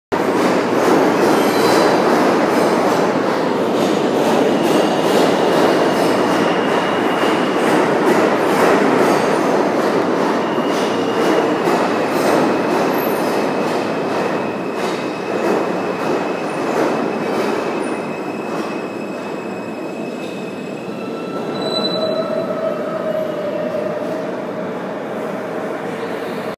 R160 R train at 59 st
I was waiting for the R train to go to Bay Ridge 77th St. and then I heard in R160 coming from the local track so I grabbed up my out my iPhone and took it